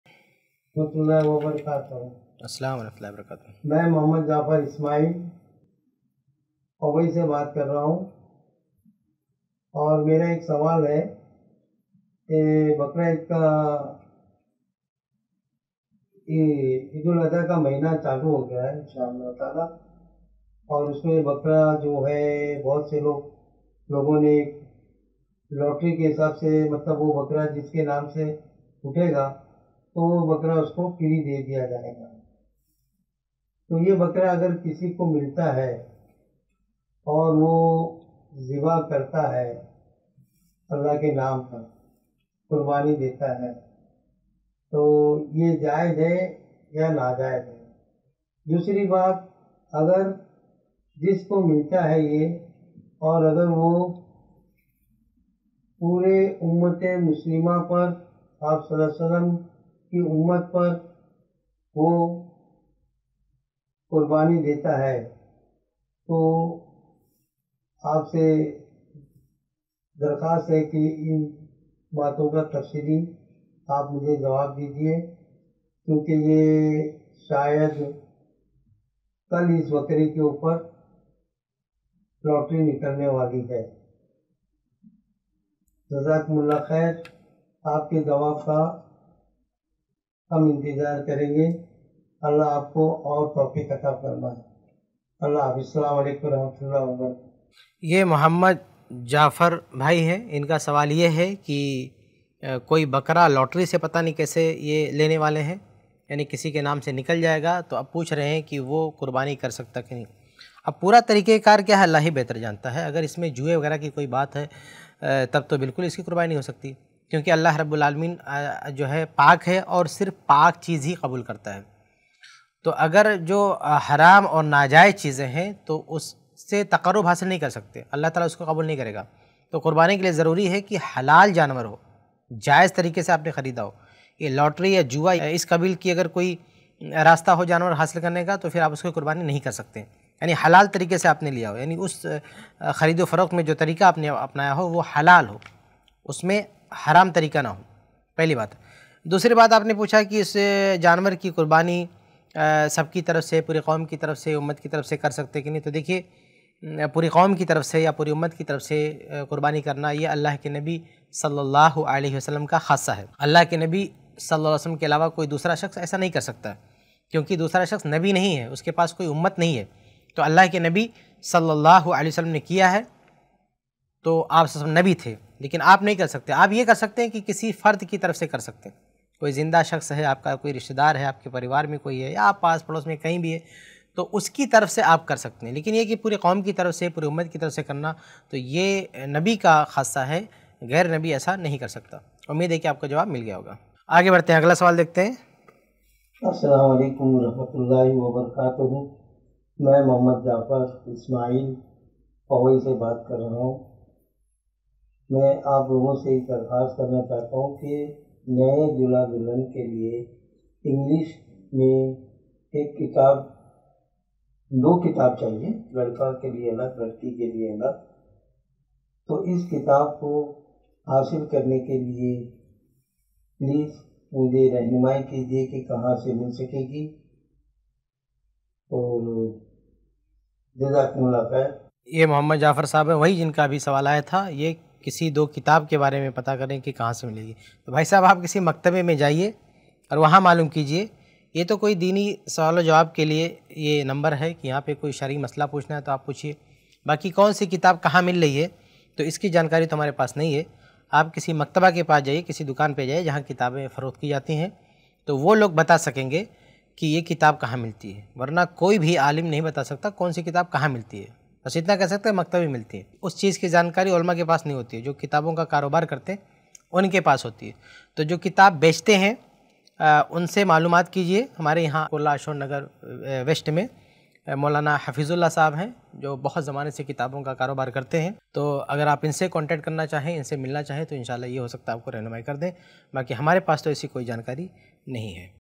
Lottery Se Nikalne Wale Janwar Ki Qurbani Karna Kaisa Hai? By Shaikh Kifayatullah Sanabili iPlus TV
Lottery Se Nikalne Wale Janwar Ki Qurbani Karna Kaisa Hai? Aur Kya Saari Ummat Aur Qaum Ki Taraf Se Qurbani Kar Sakte Hain Kya? By Shaikh Kifayatullah Sanabili iPlus TV Deeni QA

UPI : donateilm@icici